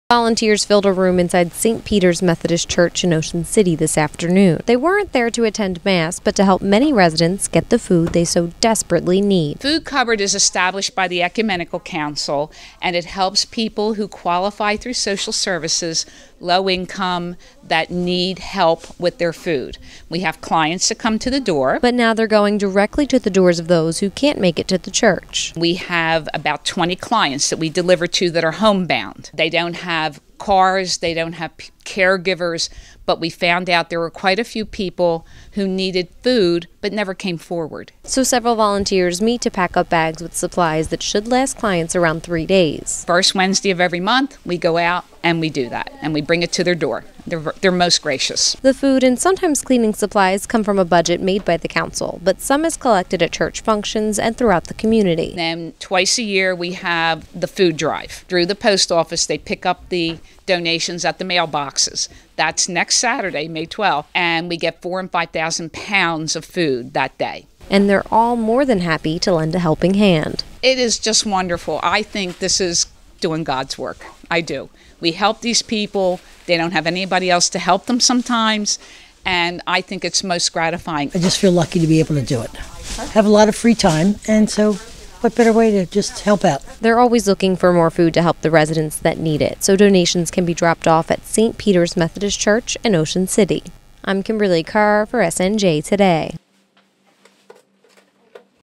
Volunteers Deliver Food to Ocean City Residents in Need | SNJ Today News
Volunteers from the Ocean City Ecumenical Council are working extra hard to make sure home-bound residents have the food they need.